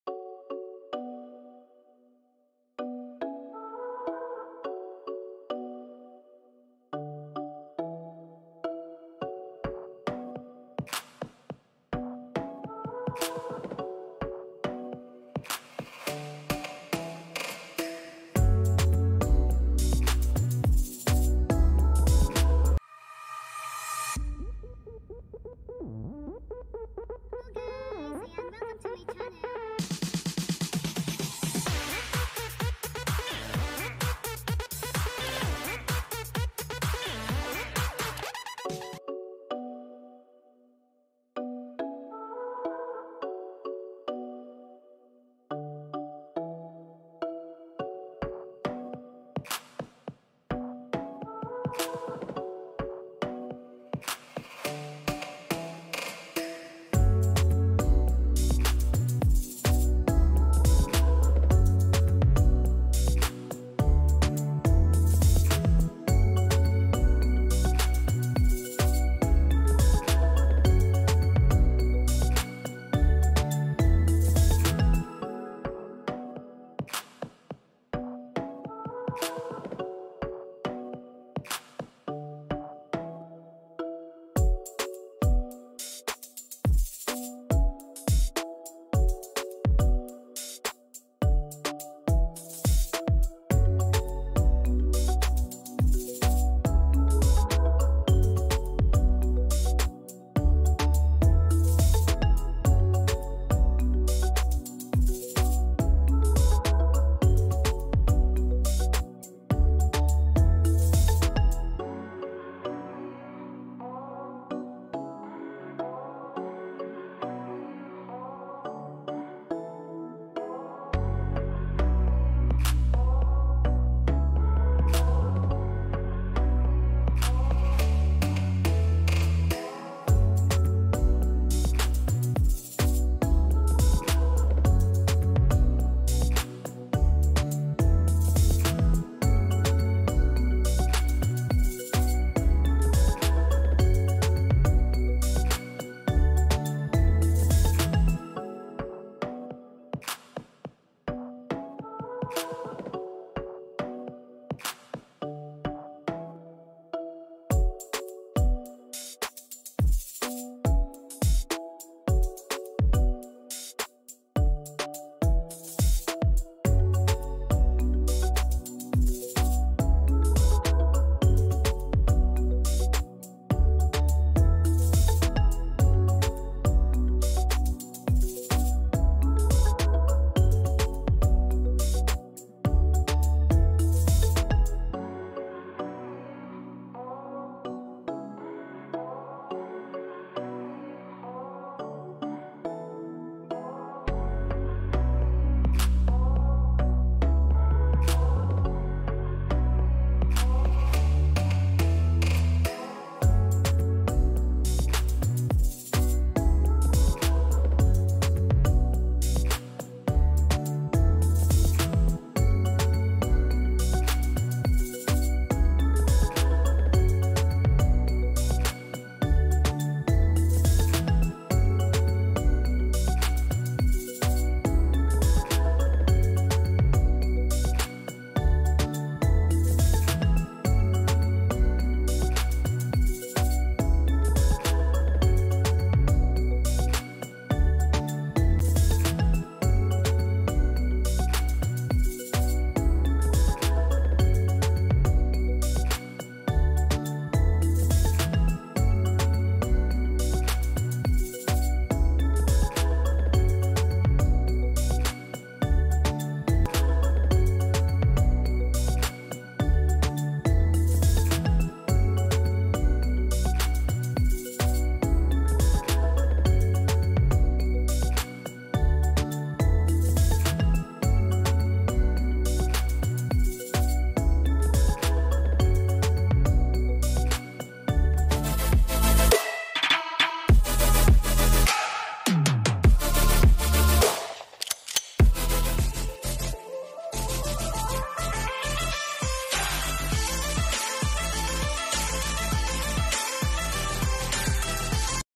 HOT & SPICY CHICKEN FEET ADOBO || QUICK AND EASY RECIPE
It's so easy to do .. it's still good for health..make it so delicious to slap